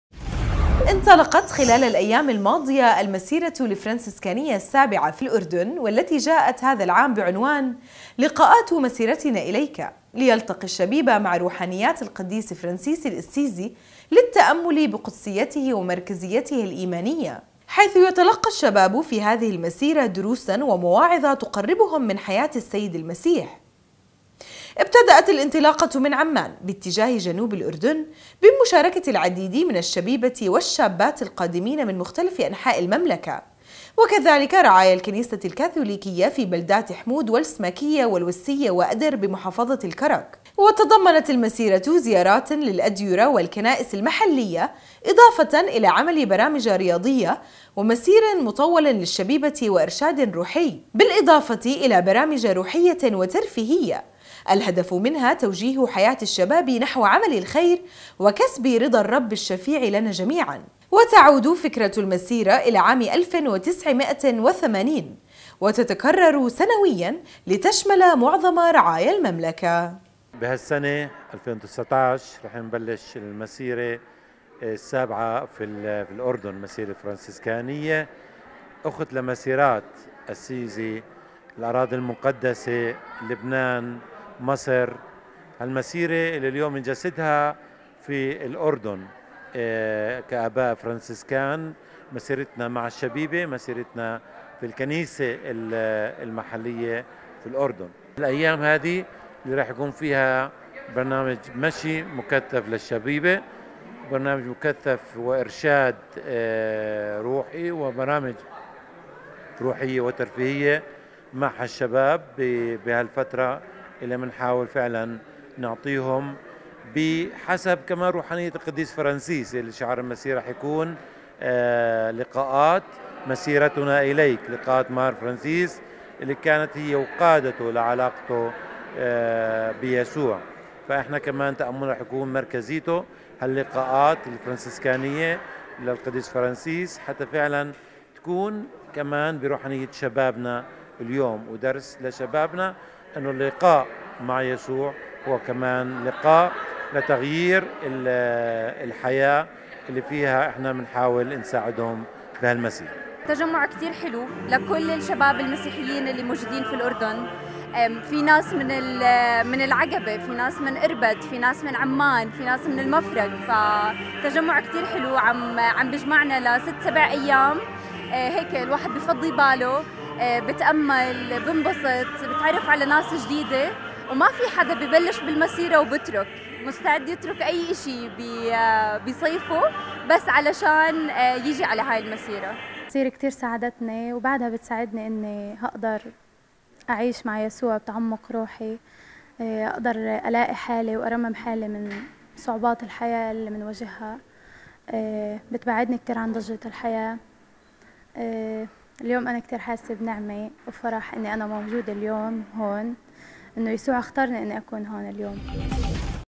نورسات الأردن : إنطلاقُ المسيرةِ الفرنسيسكانيةِ السابعةِ في الأردن
انطلقت خلالَ الأيامِ الماضية المسيرةُ الفرنسيسكانية السابعة في الأردن  والتي جاءت هذا العام بعنوان – لقاءاتُ مسيرتِنا اليكَ ليلتقي الشبيبةُ مع روحانياتِ القديس فرنسيس الأسيزي  – للتأمل بقدسيته ومركزيته الايمانية ، حيث يتلقى الشبابُ في هذه المسيرة دروسا ومواعظَ تقربهم من حياةِ السيدِ المسيح .. ابتدأت الإنطلاقةُ من عمان باتجاه جنوبِ الاردن بمشاركةِ العديدِ من الشبيبةِ والشابات القادمينَ من مختلفِ انحاءِ المملكة وكذلك رعايا الكنيسةِ الكاثوليكية في بلدات حمود والسماكية والوسية وأدر بمحافظةِ الكرك ، وتضمنت المسيرةُ زياراتٍ للأديرةِ والكنائسِ المحلية اضافةً الى عملِ برامجَ رياضية ومسيرٍ مطولٍ للشبيبة وارشادٍ روحي ، بالاضافةِ الى برامجَ روحيةٍ وترفيهية ، الهدفُ منها توجيهُ حياةِ الشباب نحوَ عملِ الخير وكسبِ رضى الربِ الشفيعِ لنا جميعا ، وتعود فكرةُ المسيرة إلى عامِ الفٍ وتسعِ مئةٍ وثمانين وتتكرر سنويا لتشملَ معظمَ رعايا المملكة .